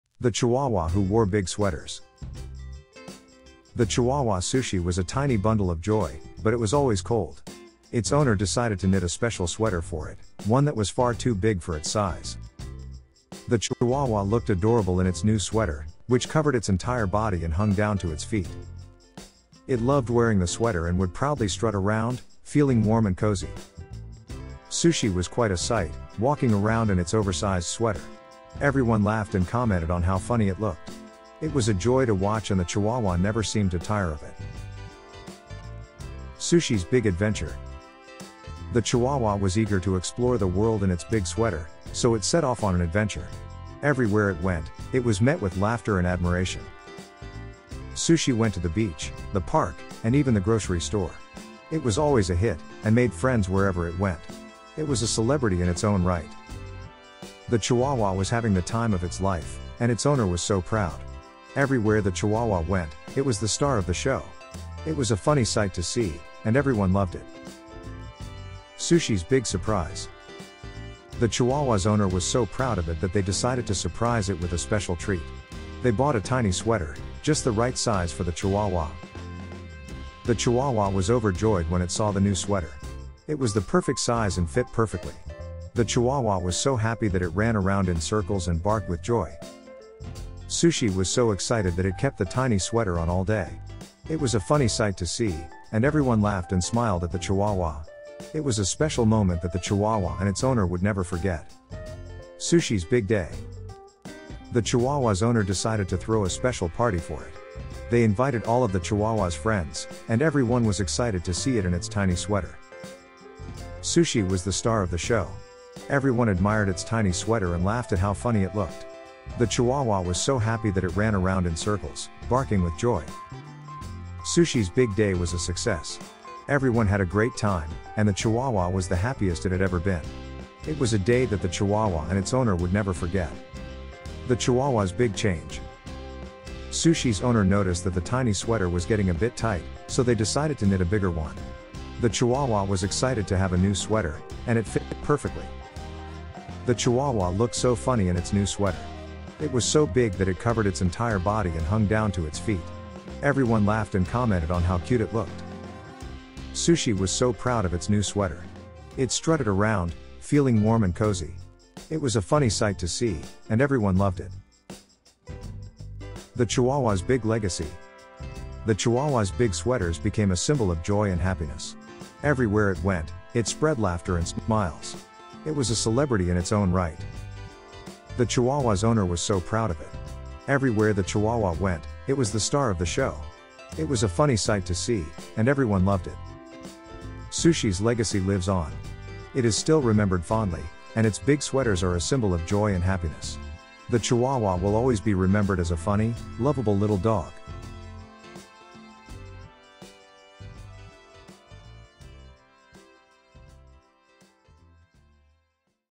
Tiny Paws, Big Style: The Chihuahua's Cozy Adventure / story for Kids
Story about Chihuahua Who wore Big Sweaters